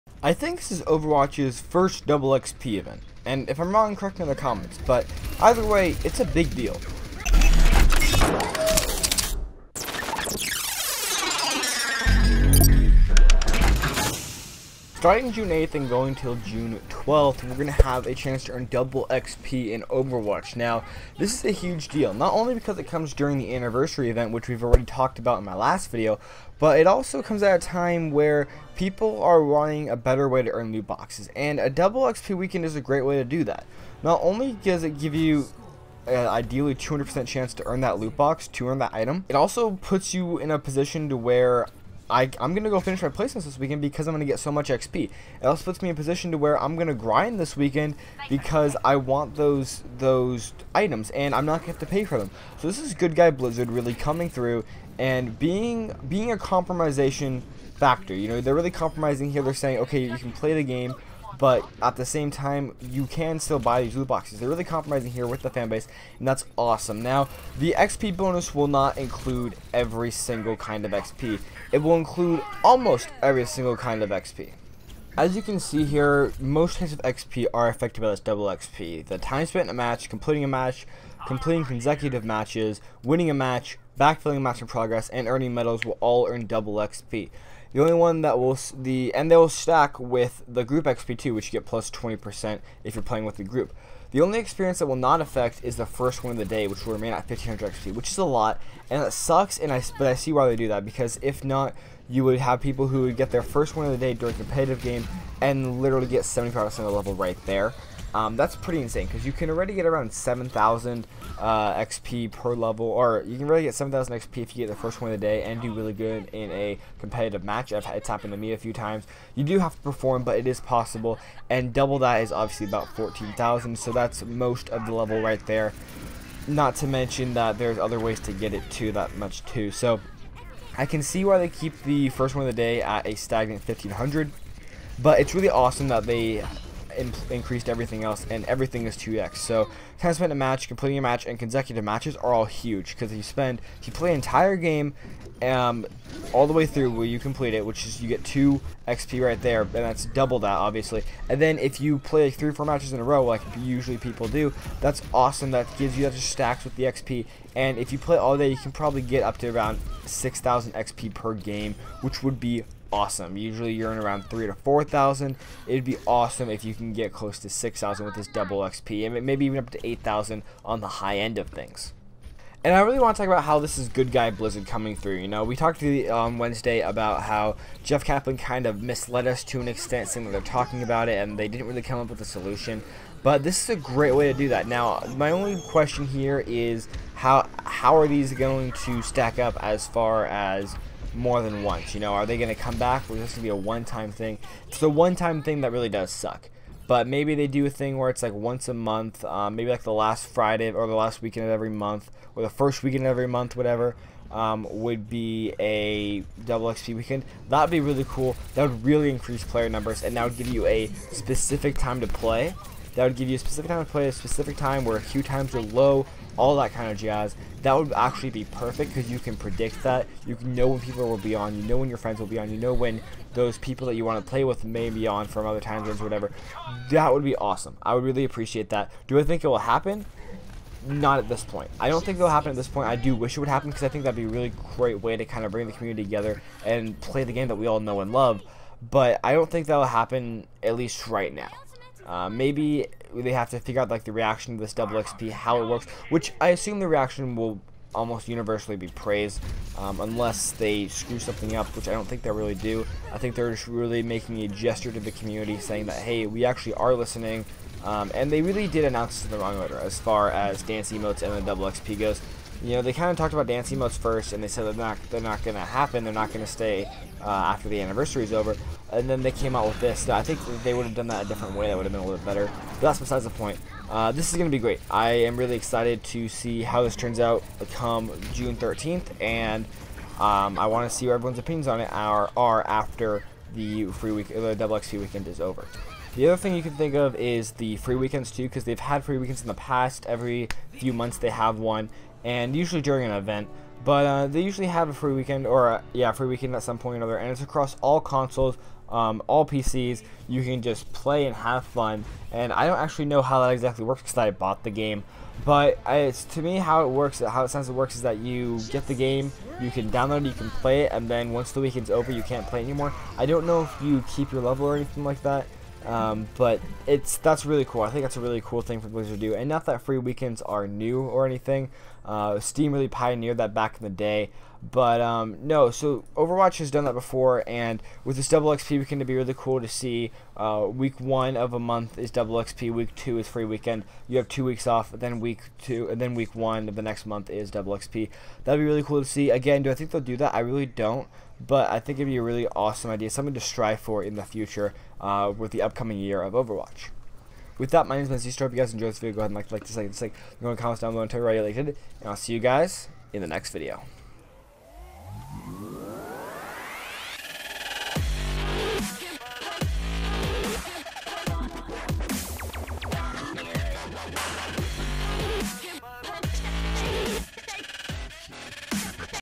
Overwatch: Double XP Event (Overwatch News & What it Means, Gaming)
Overwatch has announced its (First?) Double XP weekend and today we are going to talk about all the information we got and what it means for the future of overwatch.